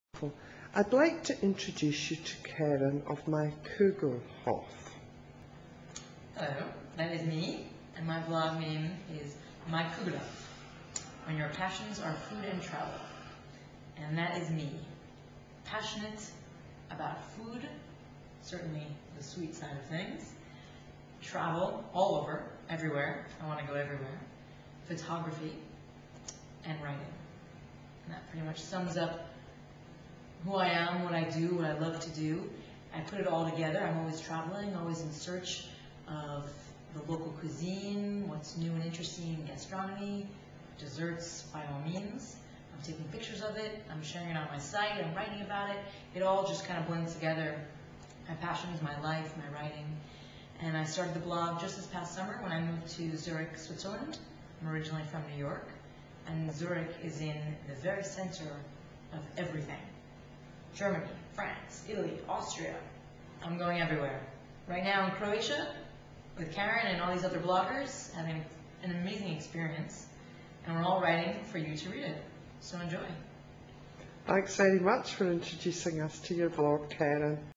Kerrin Rousset of My Kugelhopf
Kerrin Rousset of My Kugelhopf talks about her travel and food blog.